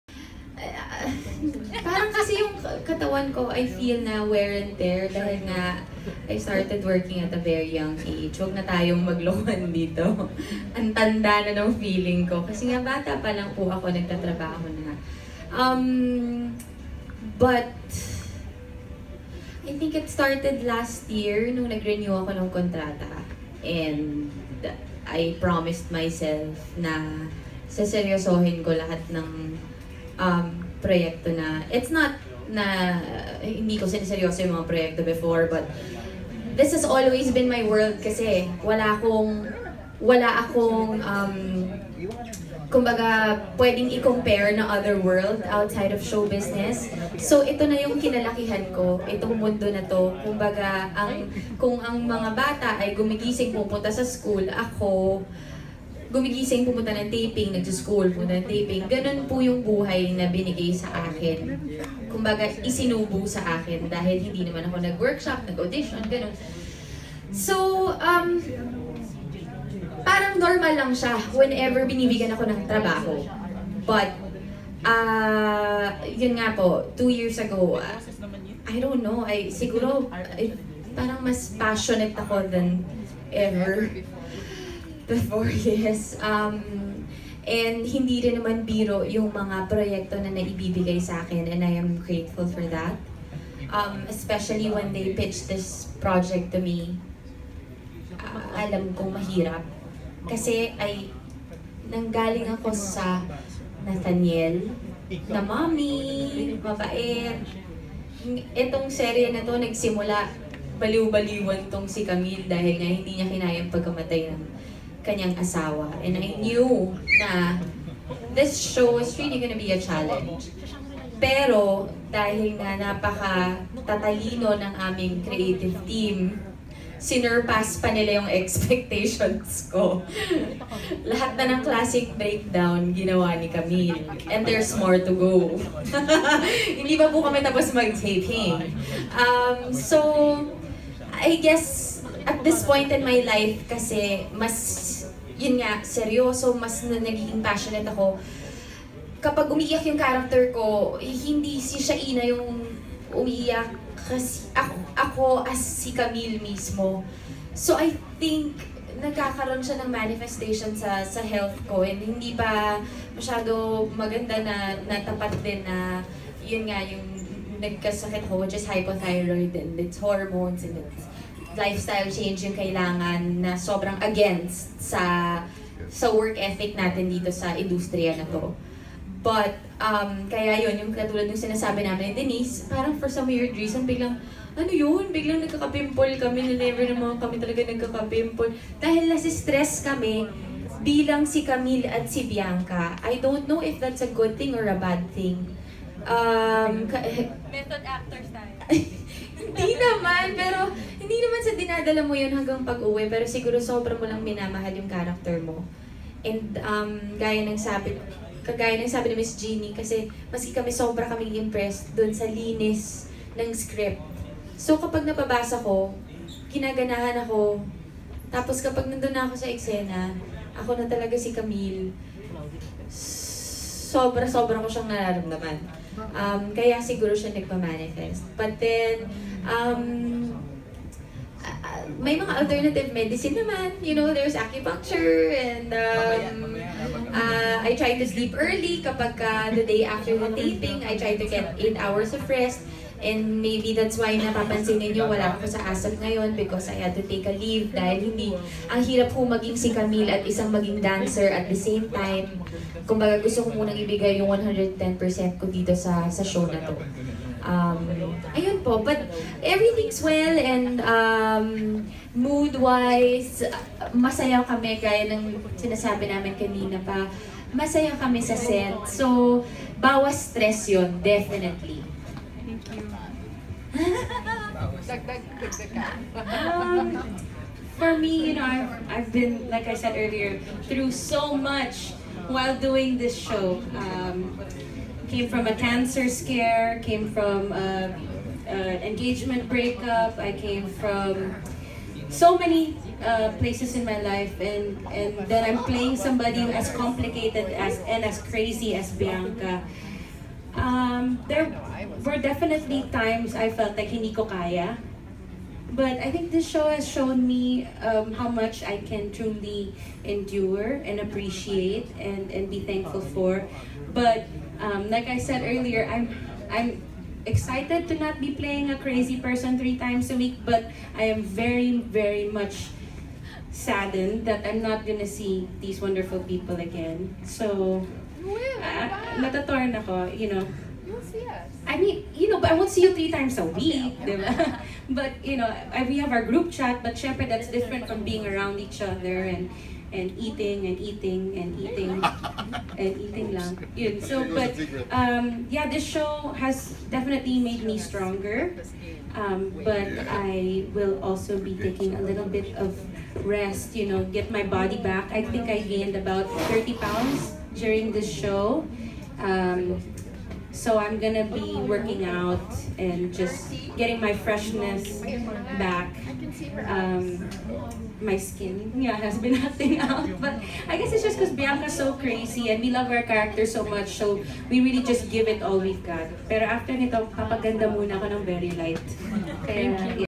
Shaina and Denise to recover from their stressful characters after The Better Half
The Better Half Finale Presscon
Shaina Magdayao and Denise Laurel previously shared how their characters Camille and Bianca have been sources of stress as they internalize their characters. Now that the teleserye is about to end, how do they plan to
recover from the heaviness of their characters?